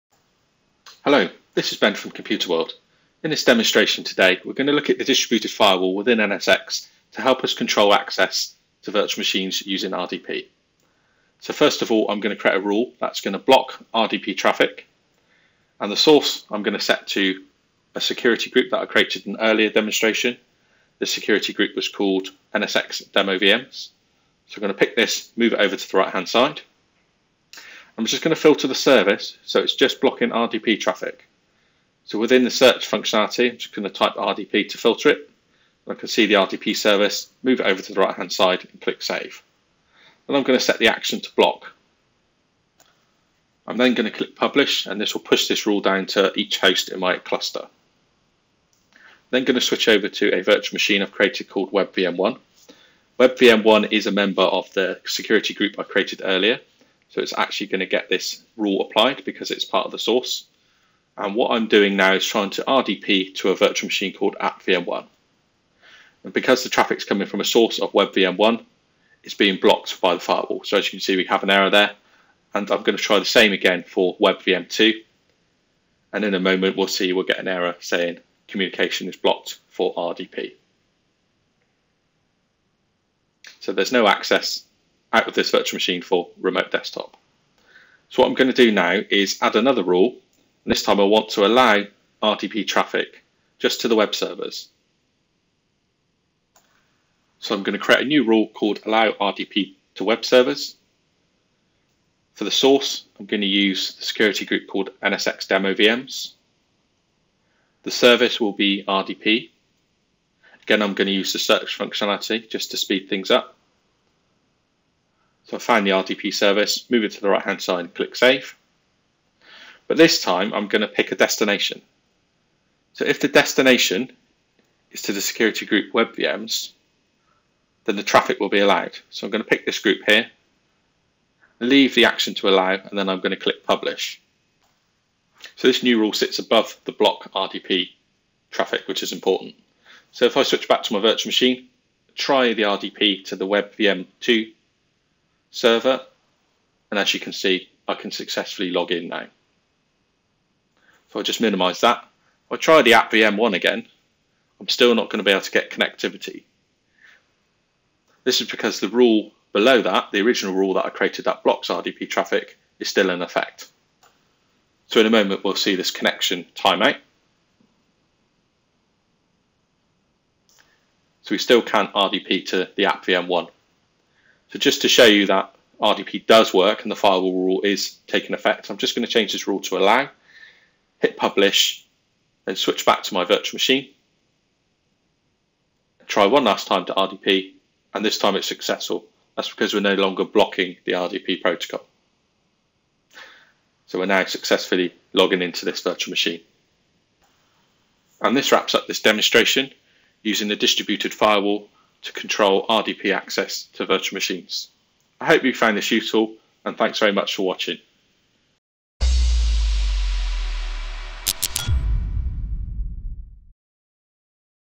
NSX Basics - Creating Firewall Rules to Control RDP Access
In this video demonstration Ben will configure the Distributed Firewall (DFW) to permit access to RDP only for a group of virtual machines. Using dynamic security groups within NSX manger allows new virtual machines meeting the criteria to be automatically protected when they are provisioned.